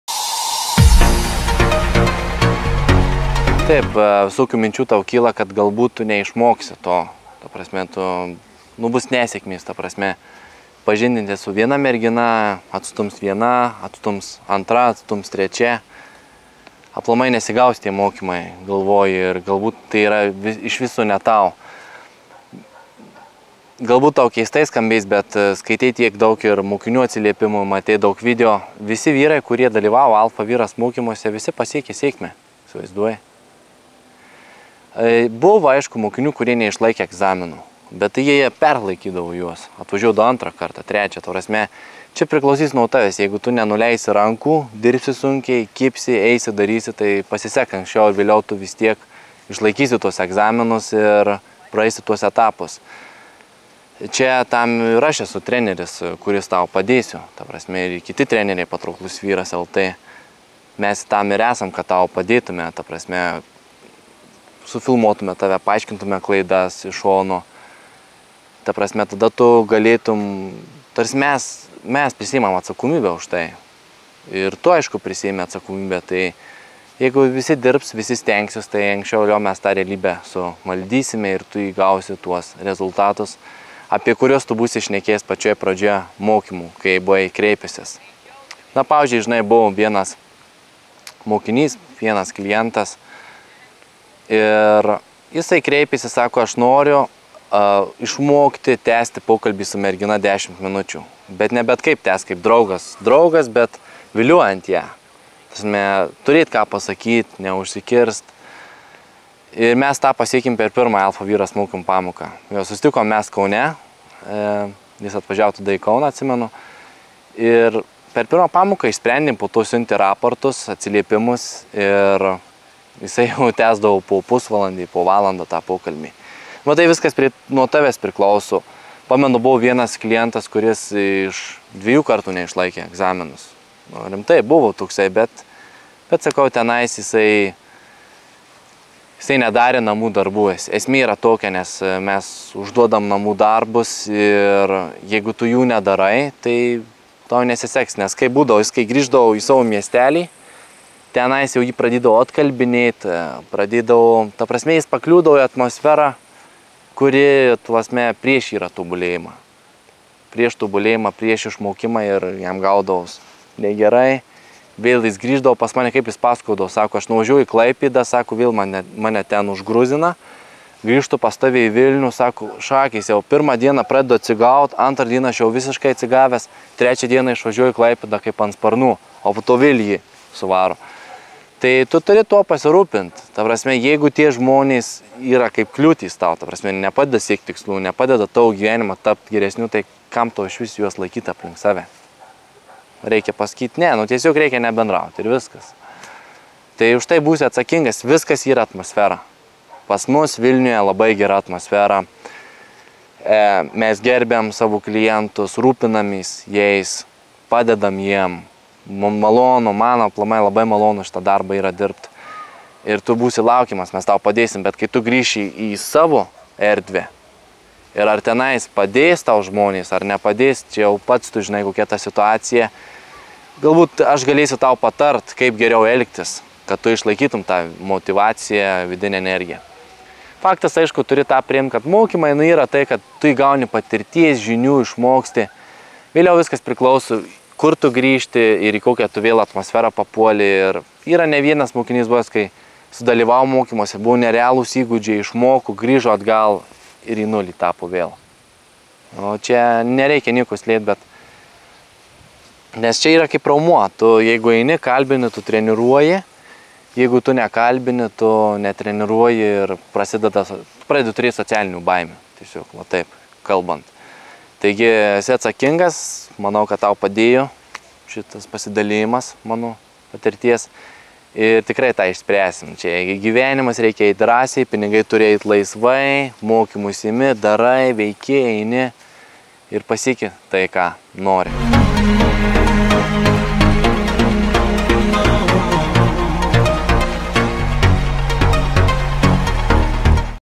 Abejoju Dėl Savo Sėkmės Alfa Vyras mokymuose
Alfa Vyras Mokymai:
Abejojantiems dėl savo sėkmės susipažįstant su moterimis ir pasitikint savimi Alfa Vyras mokymuose. Abejojantiems jog neišmoks šokti su mergina, jog nesugebės susikurti tinkamo stiliaus.